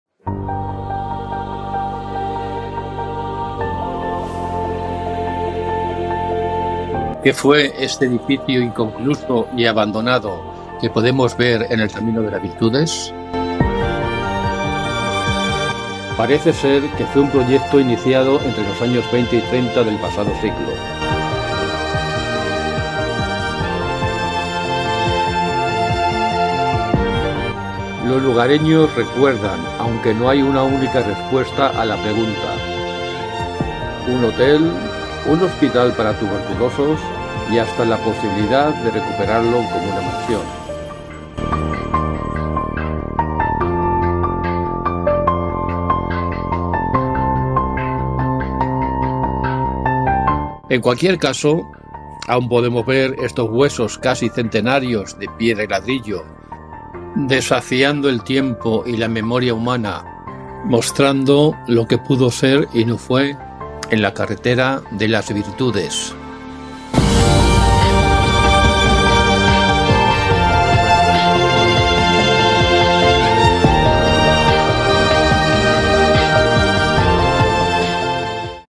Edificio abandonado
¿Un hotel, un hospital de tuberculosos, una mansión?  En cualquier caso, un proyecto que  nunca llegó a completarse. Solo ha sobrevivido este "esqueleto" de piedra y ladrillo recordándonos lo que pudo ser y no fue.
Santa Cruz de Mudela, Ciudad Real.